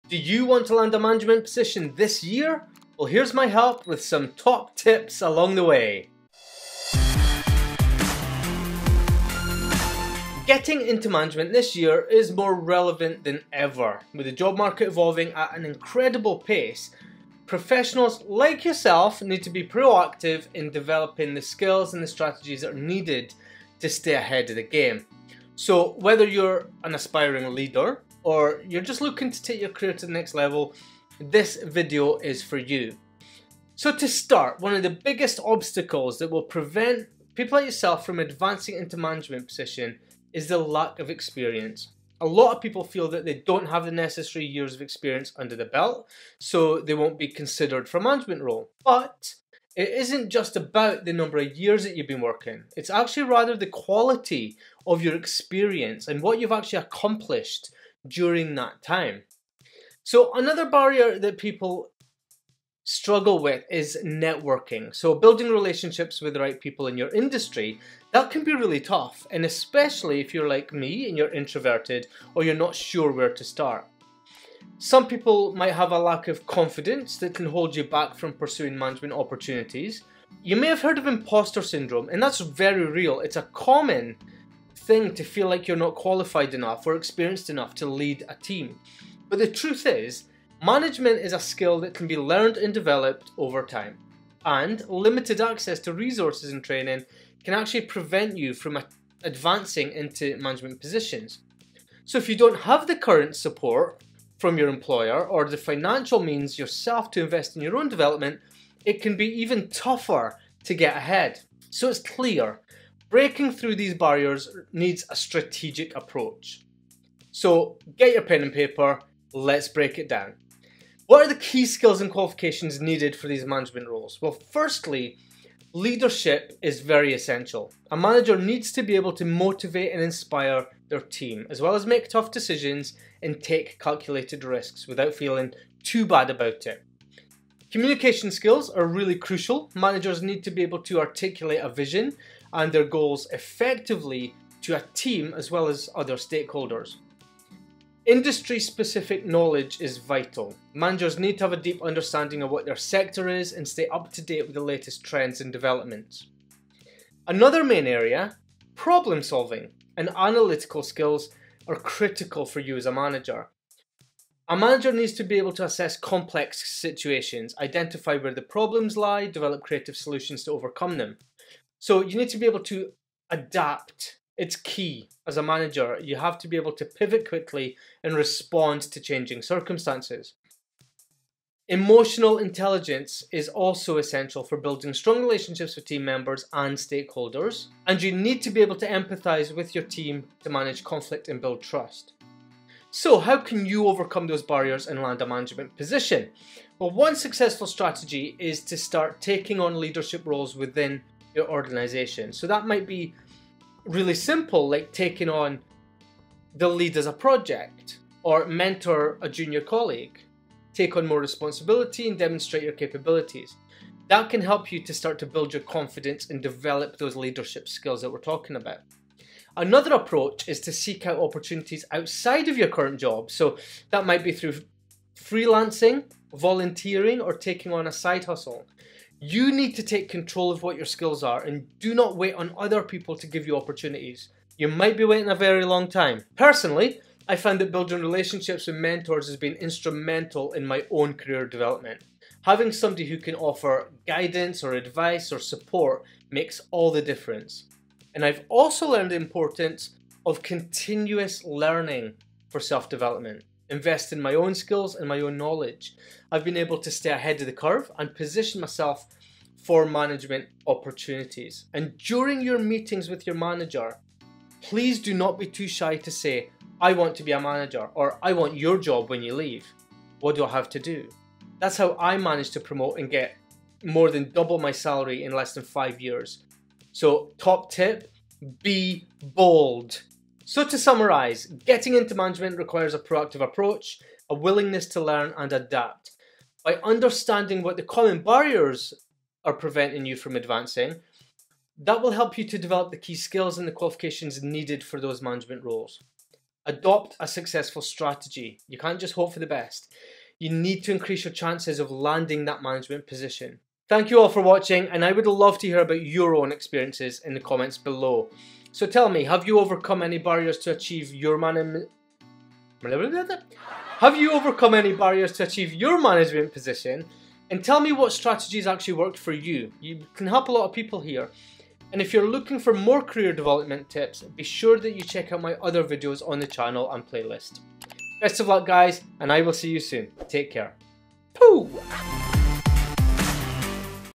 How to Get Into Management & Get Promotions | Management Skills
Are you ready to take the next step in your career and break into management this year?

In this video, we provide you with essential tips and strategies to help you navigate your way into a management position.

From understanding the key skills employers are looking for, to networking effectively and showcasing your leadership potential, we cover it all!

Join us as we discuss:

- The most in-demand management skills
- How to craft an impressive resume that stands out
- Tips for acing your interviews and making a lasting impression
- The importance of mentorship and how to find a mentor
- Steps to build your professional network within your industry

Whether you're a recent graduate or looking to advance from your current role, this video is packed with valuable insights to help you achieve your management aspirations.

**This Video Was NOT Sponsored and ALL Views are My Own**